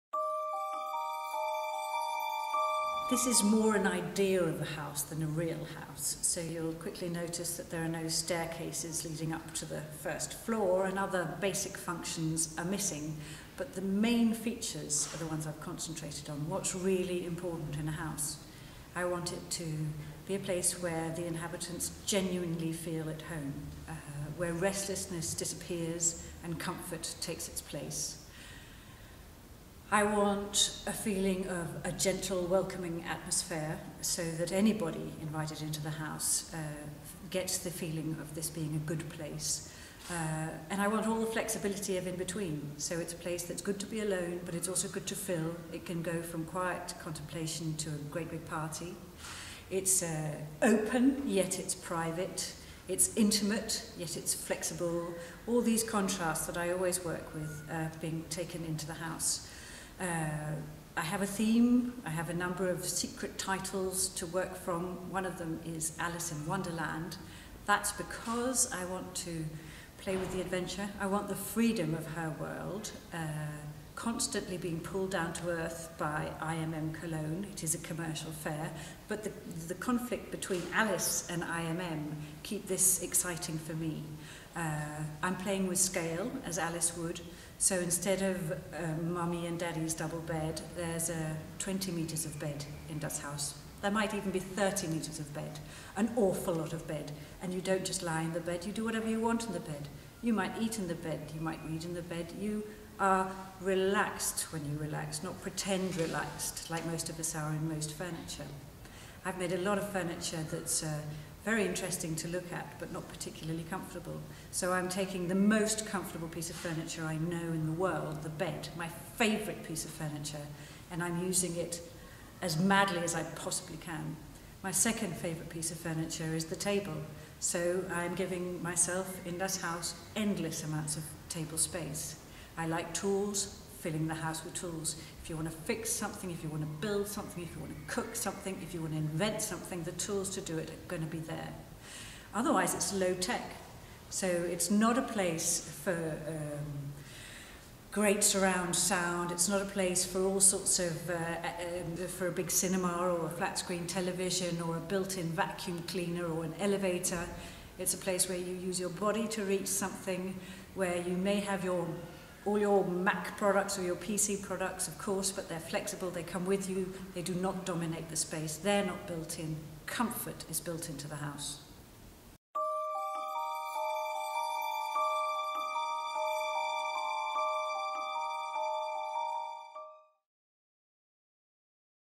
imm cologne: Louise Campbell's "Das Haus 2014" - Trailer
Das Geheimnis ist gelüftet: Neugierig und gespannt waren knapp 50 internationale Design-Journalisten nach Kopenhagen gereist, um die ersten Entwürfe vom "Haus" der bevorstehenden imm cologne in Augenschein zu nehmen. Und zwar direkt im lichtdurchfluteten Atelier von Louise Campbell, die in diesem Jahr als Guest of Honour der Koelnmesse "Das Haus - Interiors on Stage" inszeniert. Dick Spierenburg, Creativ-Director der imm cologne, präsentierte der internationalen Presse eine gut gelaunte Designerin. "Wir hatten viel Spaß beim Entwickeln der Ideen", betonte er, und freute sich, mit ihr den "Nordic New Wave" auf die imm geholt zu haben.

Den Messebesucher erwartet im Januar eine 240 qm große, zweistöckige Installation: Keine Trennung der Räume, dafür mit einem breit angelegten "soften" Areal, einem großen Arbeitsbereich, 60 Kubikmeter Stauraum und - natürlich - ausgefallenen Beleuchtungsideen. Und zum laufenden Stand der Arbeiten bemerkte sie nicht ohne Augenzwinkern: "Ich will die Freiheit ihrer Welt, während ich von der imm cologne ständig wieder auf den Boden geholt werde: Es ist eine kommerzielle Messe, aber durch den Konflikt zwischen Alice und imm bleibt es für mich interessant."

Das Haus wird innerhalb des pure village in der Halle 2.2 auf der imm cologne vom 13. bis 19. Januar 2014 zu sehen sein.